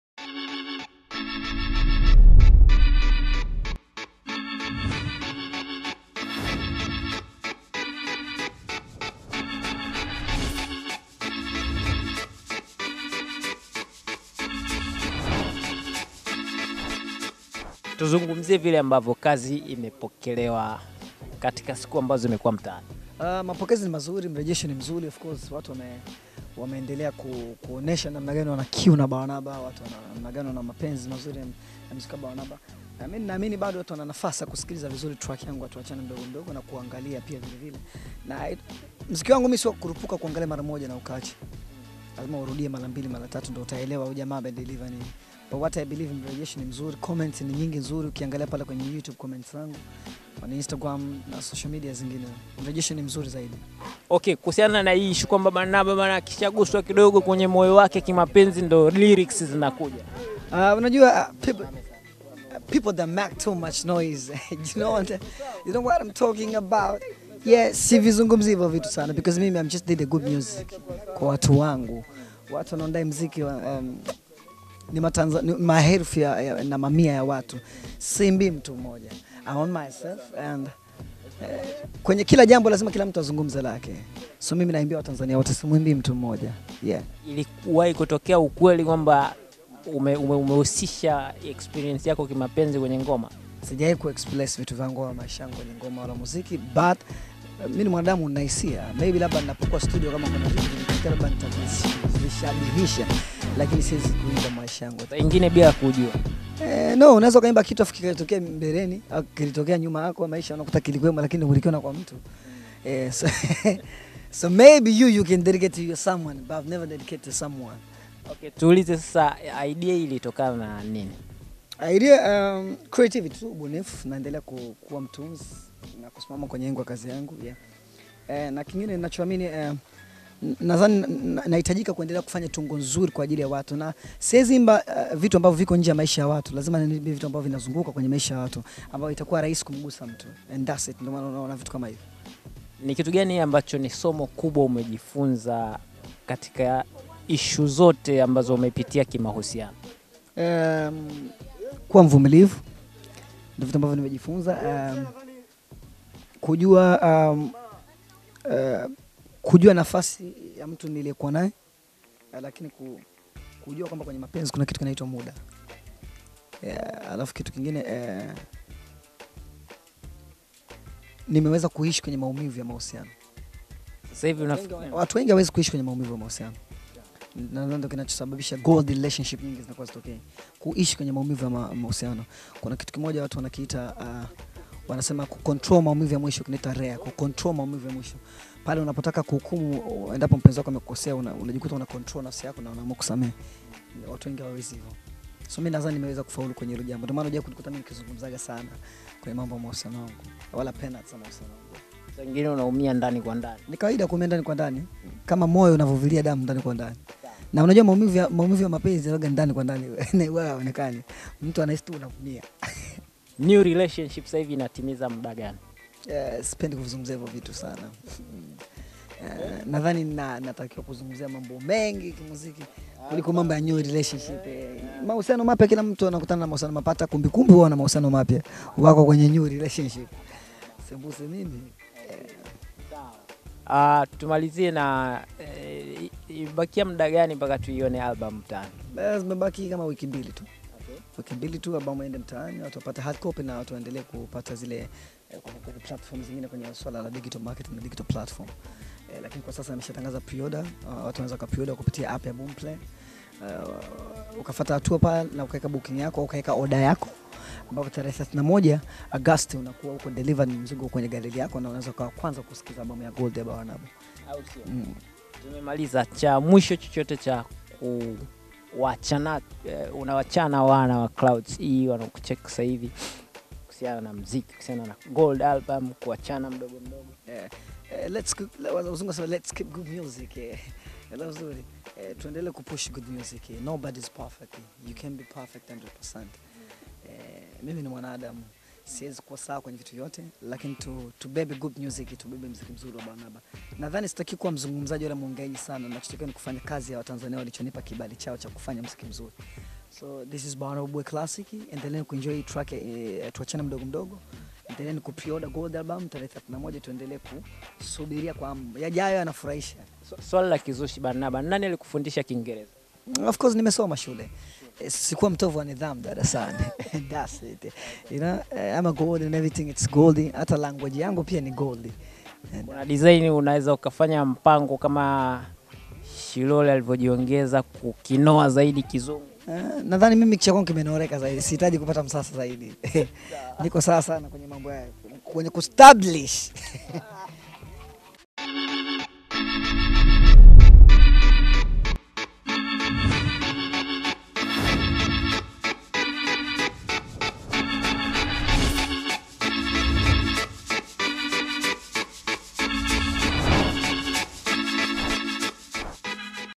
Baada ya Kuachana na Mke wake "Mama Steve", hili ndio Funzo Alilopata Barnaba Classic
Clouds E imekutana na mkali wa vocala Barnaba Classic na kusanuka haya...tazama hapa